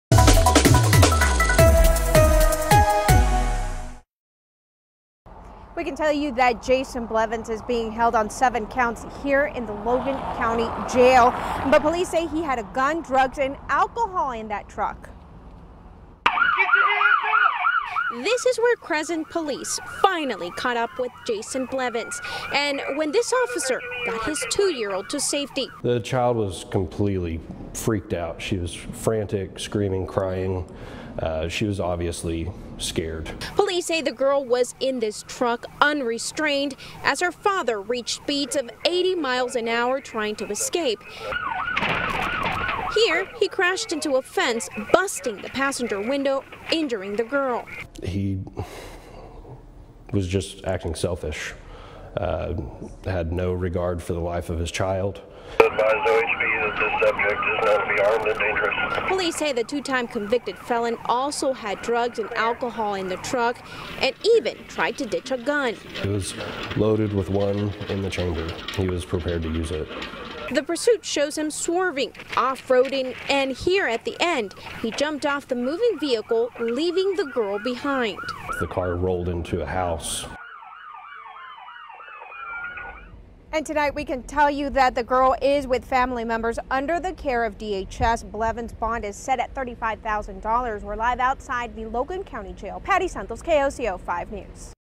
Body cam video shows wild police chase with man down dirt roads
Body camera video shows a wild police chase down dirt roads and through fields. A toddler was in the suspect’s vehicle for the whole thing, as her father, a convicted felon, tried to outrun cops.